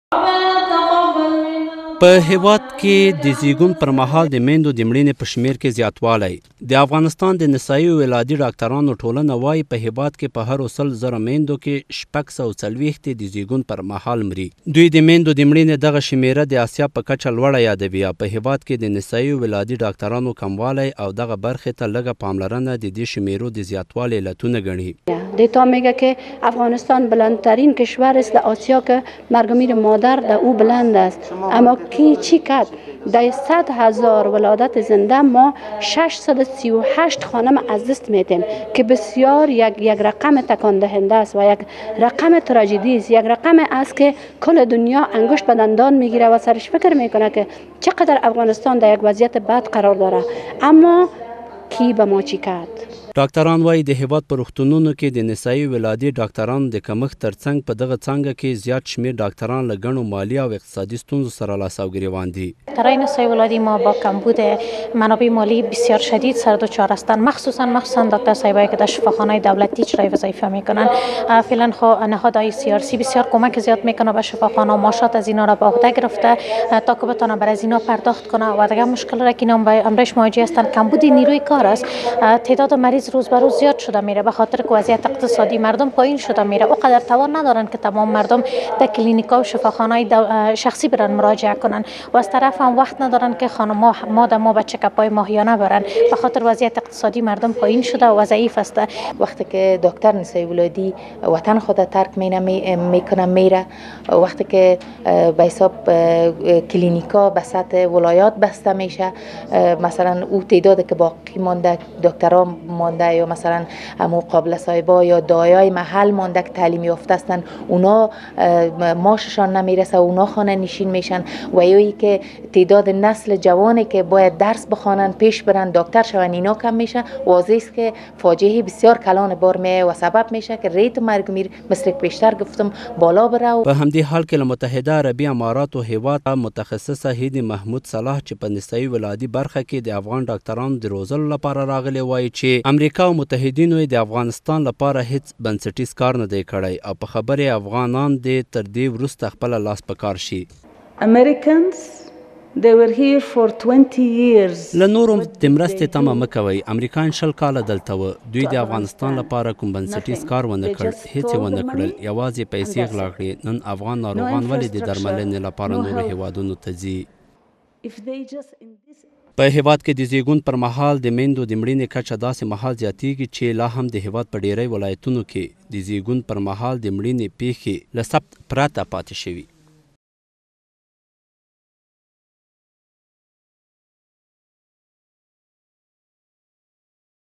په هېواد کې د نسايي ولادي ډاکترانو کمښت
د افغانستان د نسايي او ولادي ډاکترانو ټولنه وايي، په هیواد کې په هرو سل زره میندو کې شاوخوا ۶۴۰ یې د زېږون پر مهال خپل ژوند له لاسه ورکوي.